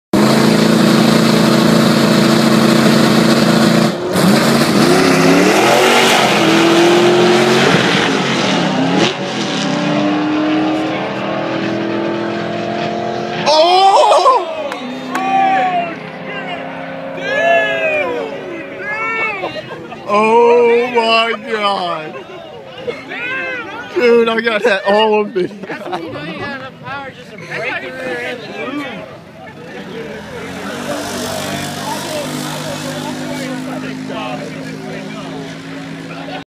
Import Face-Off 2012 Drag Truck Fail, Baton Rouge, LA
Old Chevy blows back axel off.